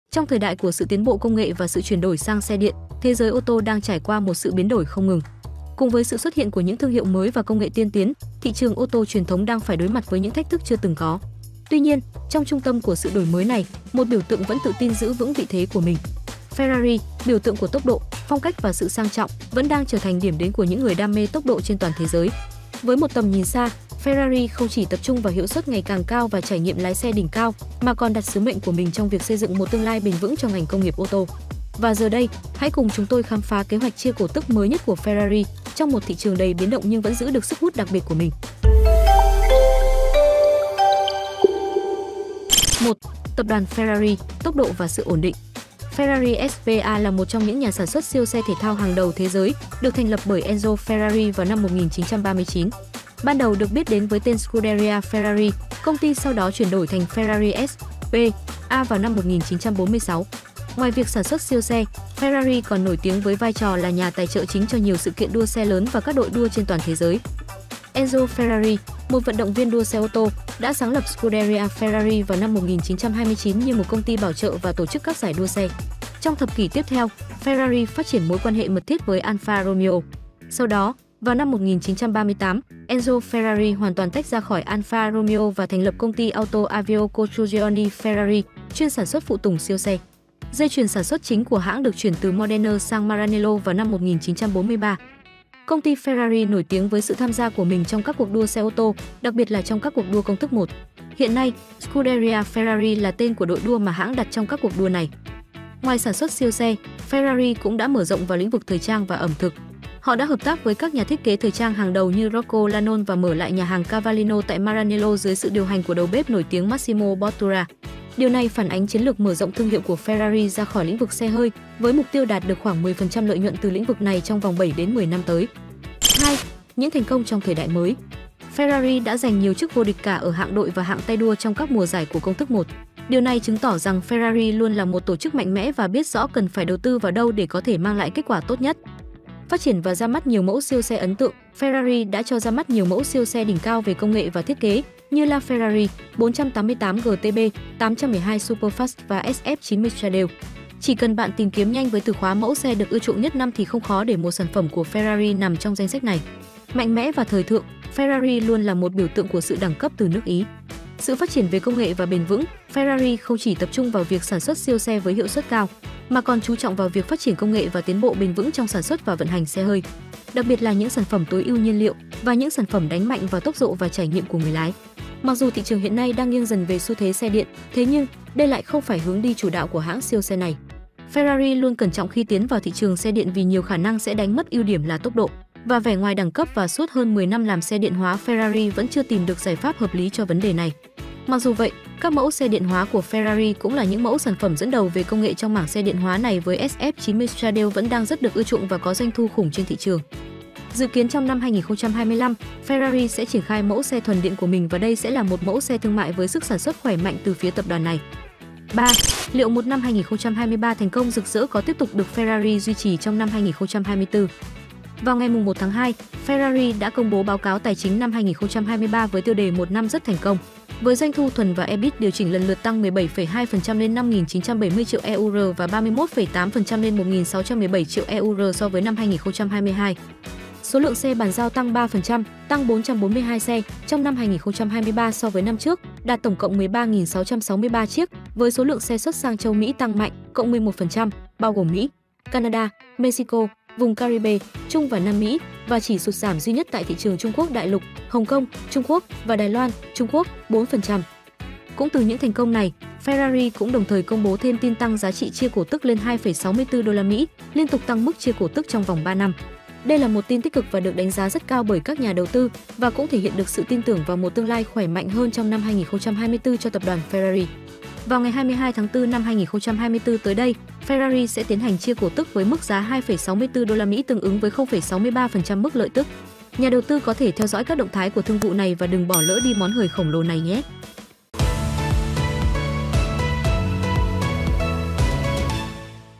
[THƯƠNG VỤ TIỀM NĂNG] THƯƠNG VỤ CHIA CỔ TỨC FERRARI - BIỂU TƯỢNG CỦA SỰ ỔN ĐỊNH - NGÀY 22/04/2024
Xem video này để tìm hiểu về thương vụ chia cổ tức của Ferrari. Đừng bỏ lỡ cơ hội đầu tư vào ngày 22/04/2024!
0:00 Intro
0:43 Tổng quan về tập đoàn Ferrari
2:10 Những thành công của Ferrari
3:36 Tiềm năng của Ferrari
🔵 The Brokers - Tra cứu, so sánh và đánh giá sàn giao dịch uy tín
▪ Top sàn Forex uy tín nhất